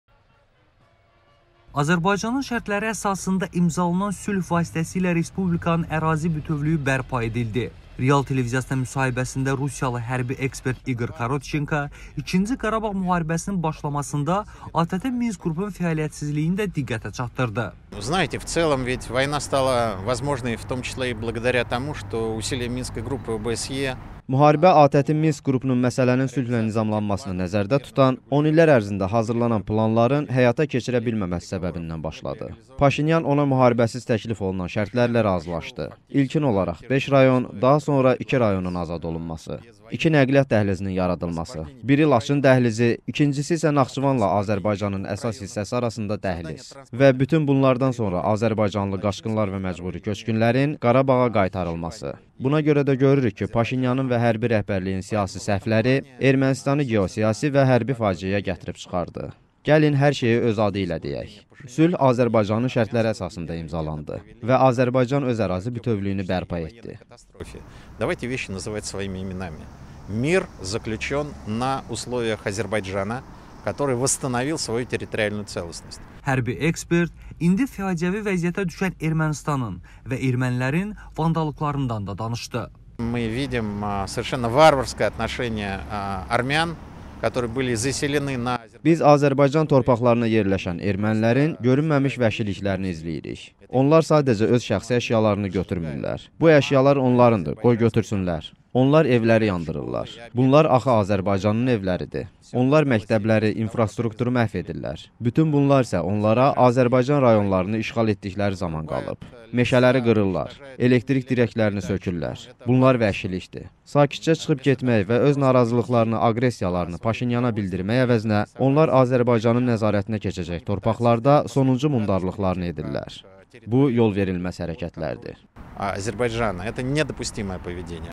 Erməni vəhşiliyinə Moskvadan reaksiya
İqor Korotçenko: “Minsk qrupunun fəaliyyətsizliyi müharibəyə səbəb oldu”